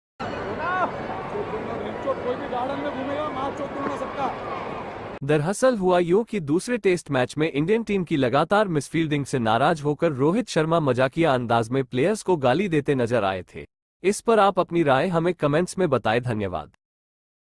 "Bc Maa Ch** dunga bc" |Stump mic caught Rohit Sharma abusing his teammates
Rohit Sharma abused his teammates during the 2nd test match which was played between India vs England in which Rohit was saying that Maa chod dunga behenchod jo bhi Garden me ghumta dikha Maa chod dunga and he got caught at the stump mic.